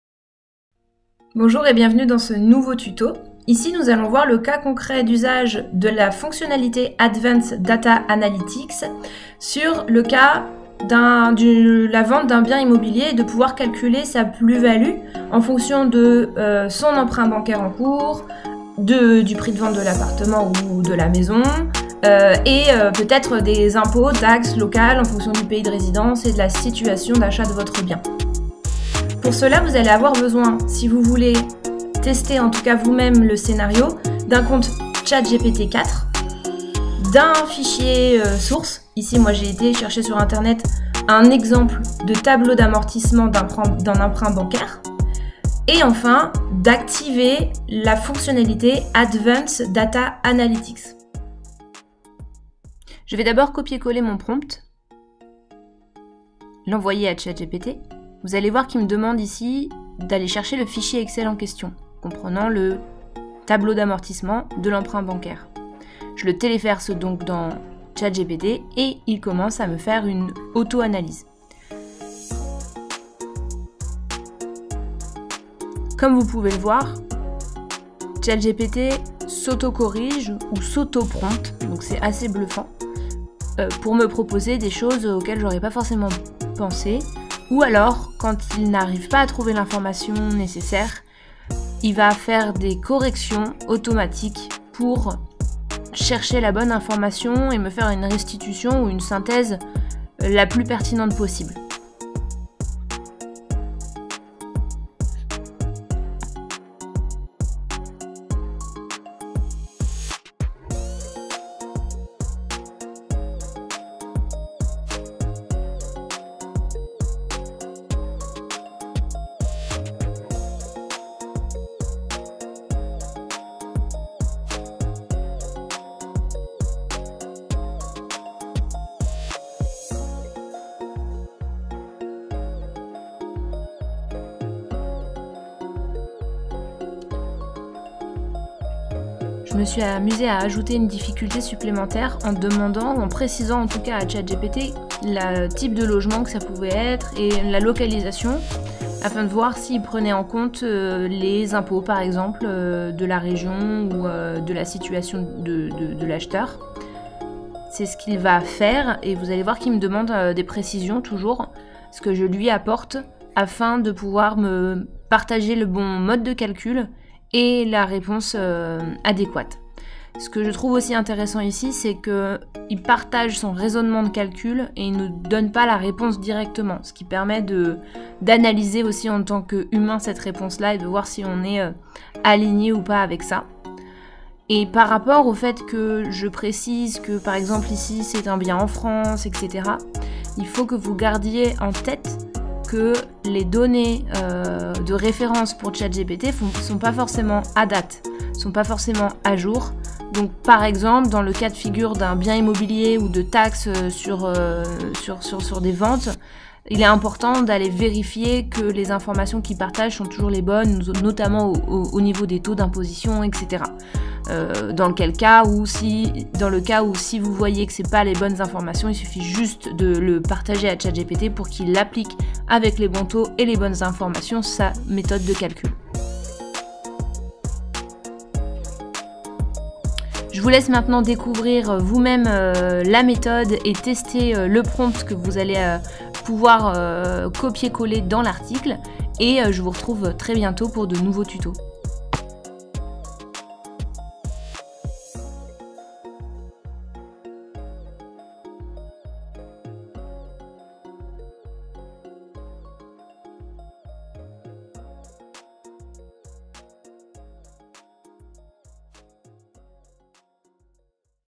Transformer ChatGPT en conseiller financier - Analyse d'un tableau d'amortissement - immobilier
Découvrez comment ChatGPT peut vous aider à calculer l'argent disponible lors de la vente d'un bien immobilier avec un emprunt en cours grâce à l'analyse d'un tableau d'amortissement.